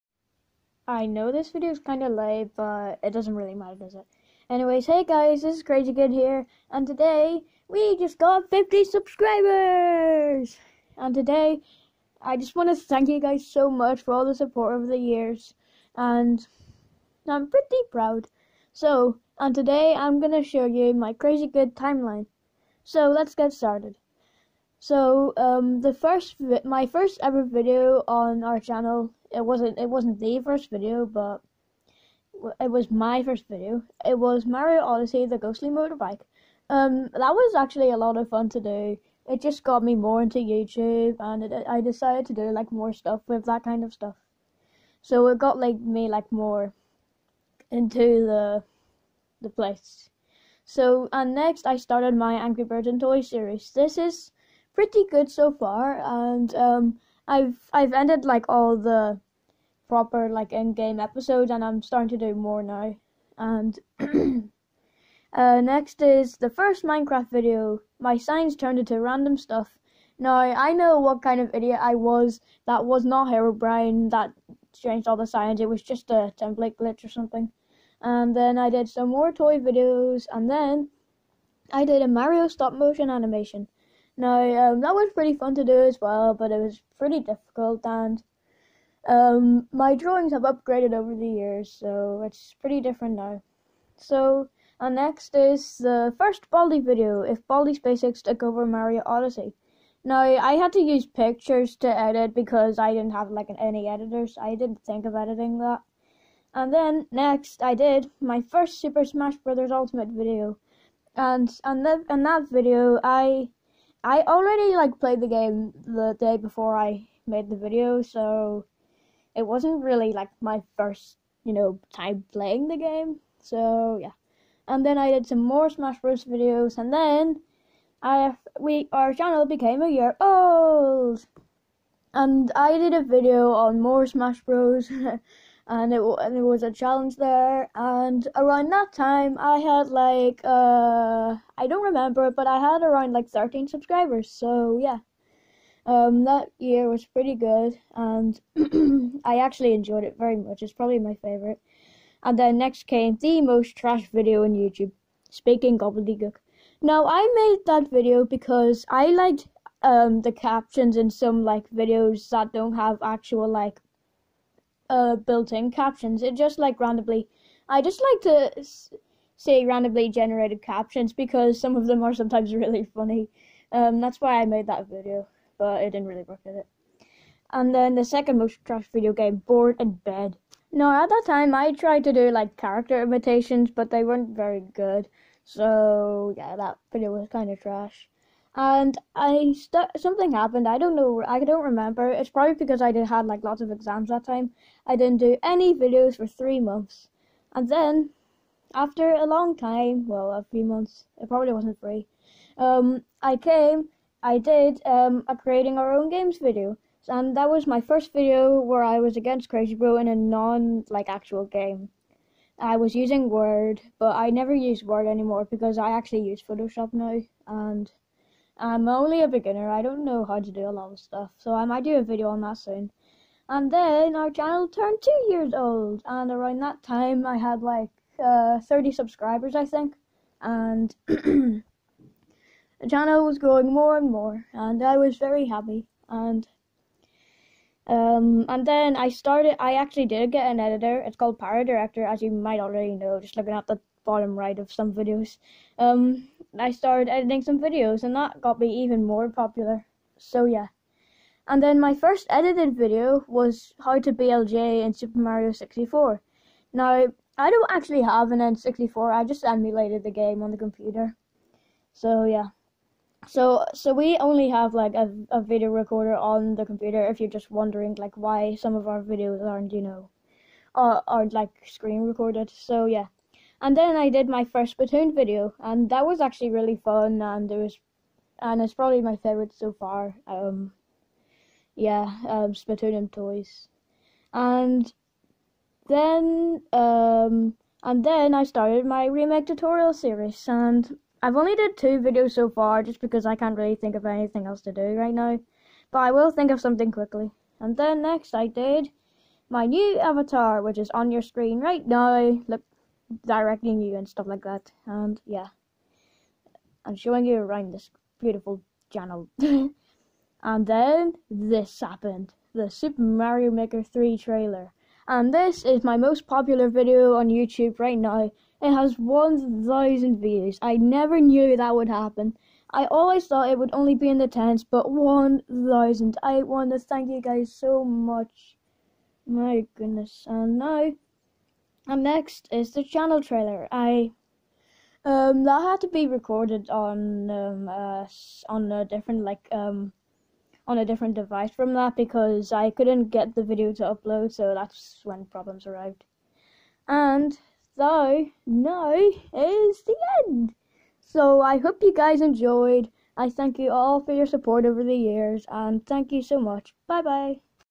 Crazykid's 50 Sub Special
Crazykid is showing his channel timeline!